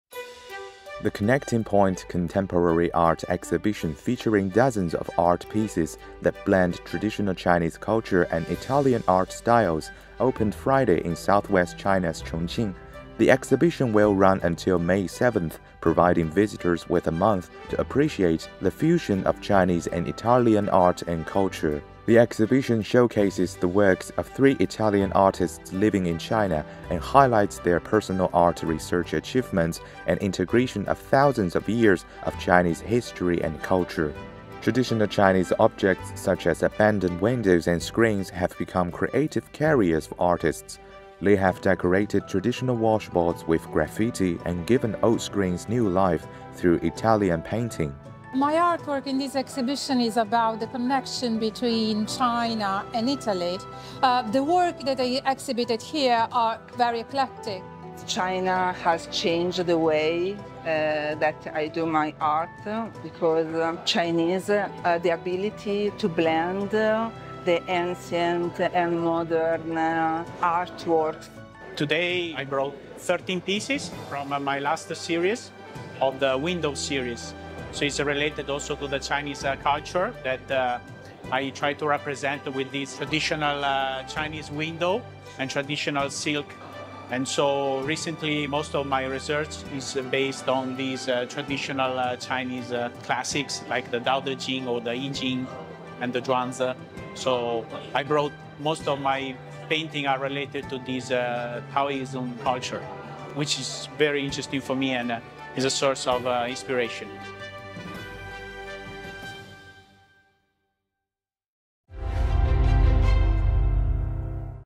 GLOBALink | Art exhibition showcasing fusion of Chinese, Italian cultures held in Chongqing
The "Connecting Point" contemporary art exhibition, featuring dozens of art pieces that blend traditional Chinese culture and Italian art styles, opened in SW China's Chongqing.